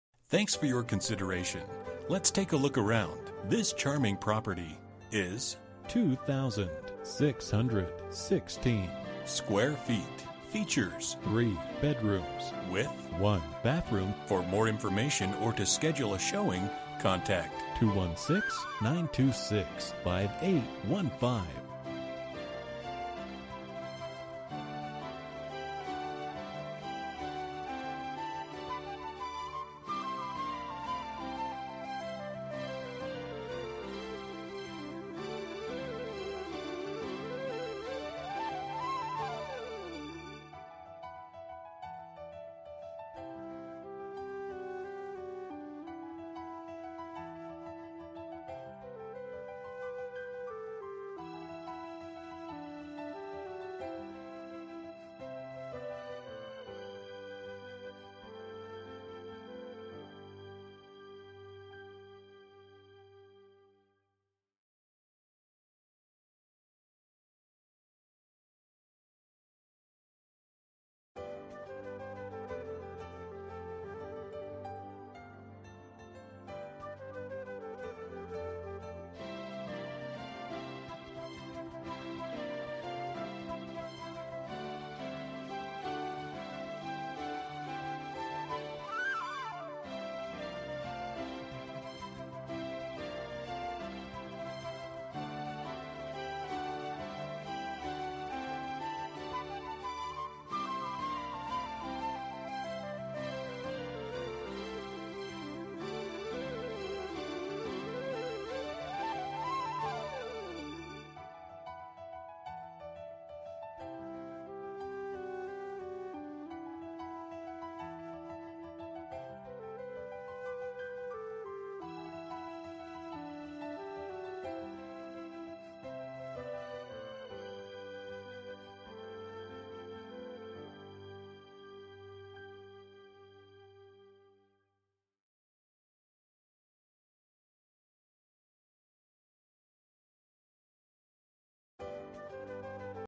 Home for sale in North Olmsted, OH - 4464 Dover Center Road
Lovely, large trees in the front yard provide shade and privacy for this gorgeous home that has a convenient u-turn driveway. Outdoor lovers will love the large yard that has a garden and a pond that can be viewed from the enclosed porch. A storage barn provides extra storage. Views of the gorgeous yard can be seen through the newer Anderson windows. The house boasts an updated bathroom and kitchen complete with newer appliances. All of the rooms have freshly, updated paint. Newer Roof, A/C and furnace (2012).

Bedrooms: 3
Bathrooms: 1
Square Feet: 2616
Price: $183,000